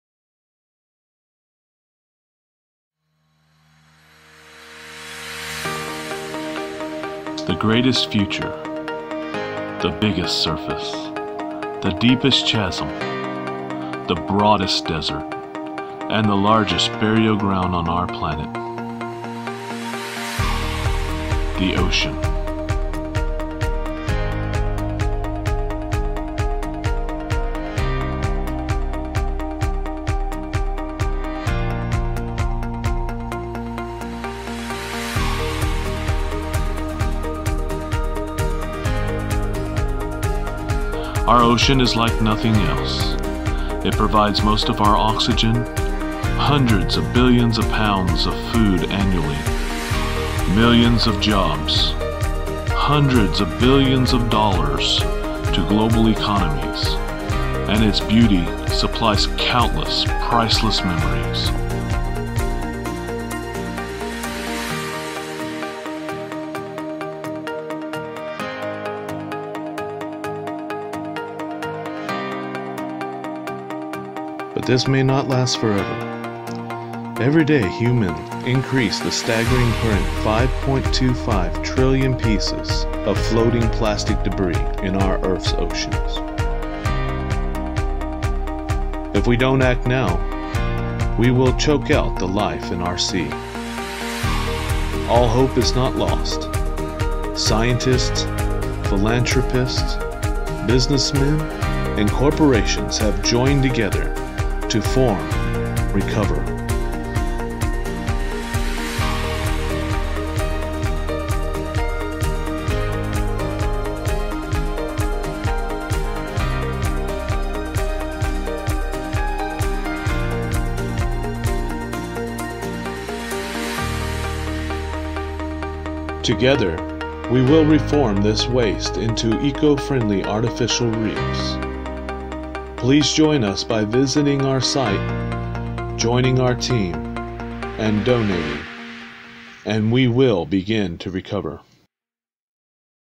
Clean our Ocean | RECOVER
The average American produces 102 tons of garbage in one lifetime. With currently over 537,194 square miles of floating plastic, the days of collecting with nets, or picking up by hand are over. We must come up with a legitimate method to gather and eliminate this problem.The numbers are staggering: There are 5.25 trillion pieces of plastic debris in the ocean. Of that mass, 269,000 tons float on the surface, while some four billion plastic microfibers per square kilometer litter the deep sea. Scientists call these statistics the "wow factor" of ocean trash. The tallies, published last year in three separate scientific papers, are useful in red-flagging the scope of the problem for the public, but beyond the shock value, just how does adding up those rice-size fragments of plastic help solve the problem? Although scientists have known for decades about the accumulating mass of ocean debris and its deadly consequences for seabirds, fish, and marine animals, the science of sea trash is young and full of as-yet unsolved mysteries.The rate of trash being fed into our ocean vs the amount of trash being removed using non feasible old ideas is increasing every day. RECOVER will turn the tide. No more collection with a net just to make an agonizing trip to return the waste to the landfill it originated in. We will turn that waste into usable eco friendly reefs that will promote life in our once clean oceans. RECOVER's system is brilliantly simple. Utilizing off-shore supply ships, fitted with our patented process, we collect the floating trash at the front of the ship, convey it to the rear, into our injection molding machines, that turn this once hazardous material into patented Ersatz Chain Links. These Links are then either connected and dispensed off the rear of the ship, to form fresh environments for sea life to flourish, or they are transferred to locations to be used as barrier Ersatz Reefs. Ersatz (Artificial) reefs benefit ocean sea life, giving a place for coral to form. These areas begin to flourish with sea life, which aids in creating a new and brilliant eco-system. The Reef can also be relocated to coastal areas that are suffering from land tidal erosion. Not only can ocean currents be redirected to save coastal lands but these newly formed Ersatz Reefs will attract sea life making the coastal areas attractive to many, including oceanography researchers, sports fishing, or sea life observers.